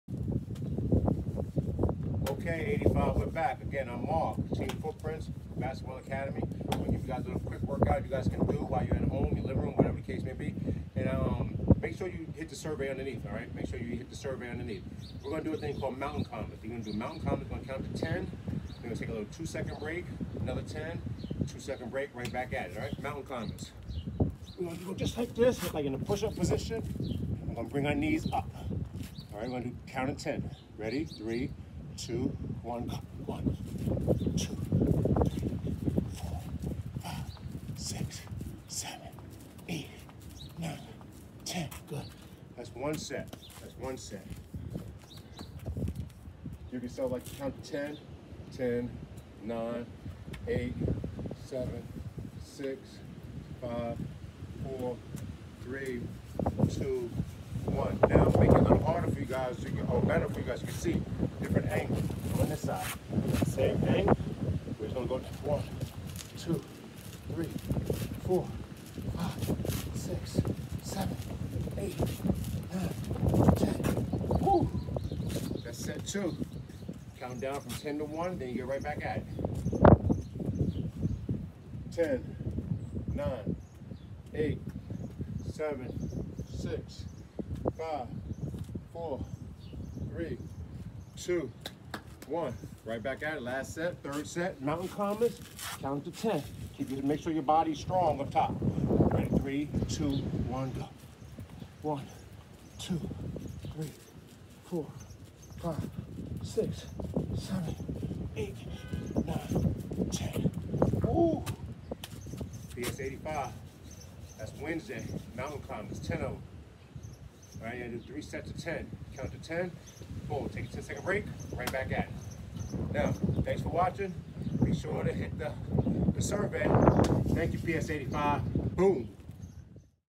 FOOTPRINTZ HOME WORKOUTS via FORDHAM
PS 85 (WEEK 1 WEDNESDAY)

This workout, consists of exercises that students can perform, using only your body weight for resistance and in a small space which can be either your house, backyard, and/or park.

The goal is to complete as many good-form reps of each move as you can in as short a time as possible. Students can modify the moves to make them easier or harder as needed, and adjust their pace depending current fitness level.  IDEALLY.

10 count mountain climbers. (3 sets) - WEDNESDAY